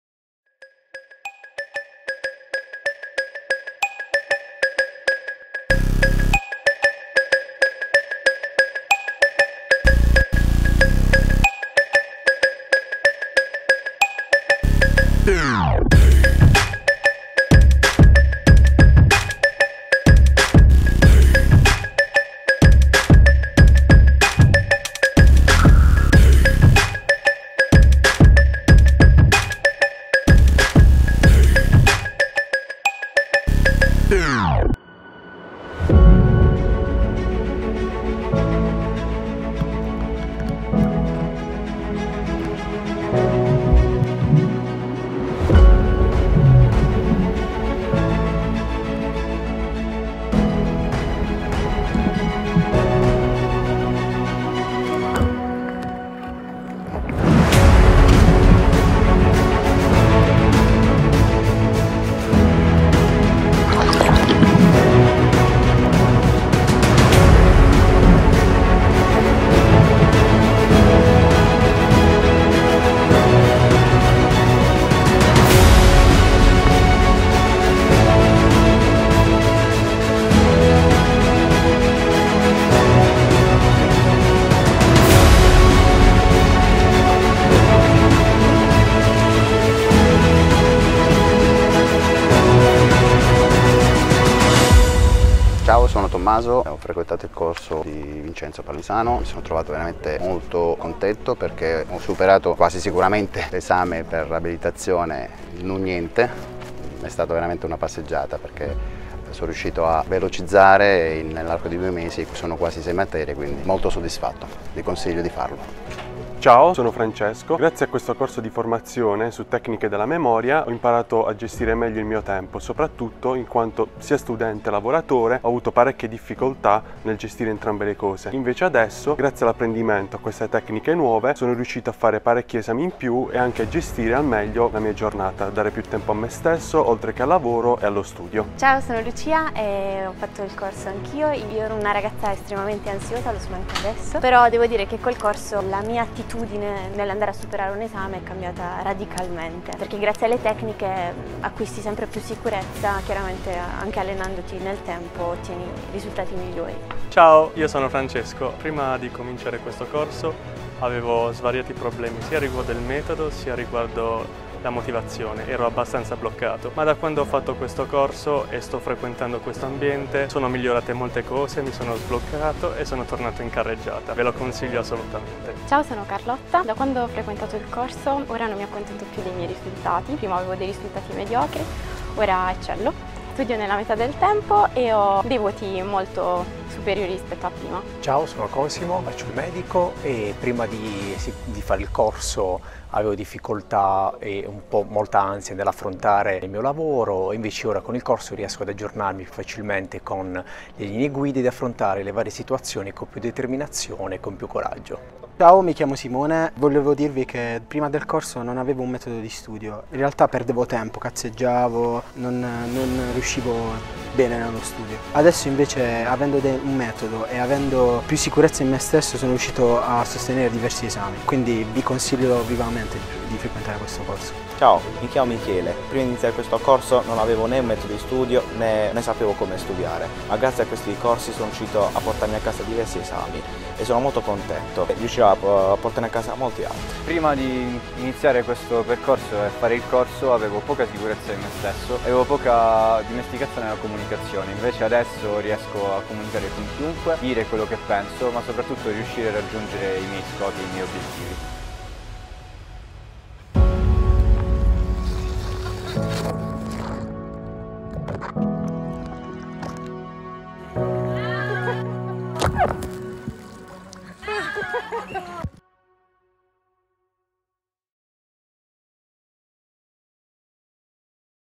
Ecco che sfida ho sottoposto ad alcuni miei studenti
Ho chiesto ad alcuni miei studenti di memorizzare dei numeri molto complessi in assenza di ossigeno.
Esperimento fatto per divertirci un po' e giocare con le tecniche di memoria.

Ovviamente loro usano le tecniche per cose bene meno frivole... vedrai i loro risultati nel video.